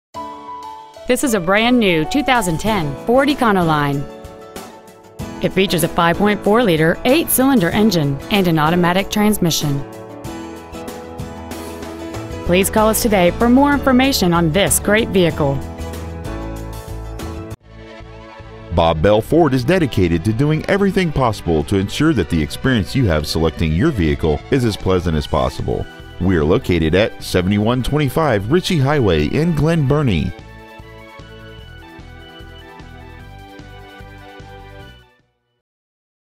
2010 FORD ECONOLINE 350 MD
Year : 2010

 Make : FORD

 Model : ECONOLINE 350

 Engine : 5.4L V8

 Trans . : AUTO

 Miles : 6

 7125 Ritchie Hwy

  , MD 21061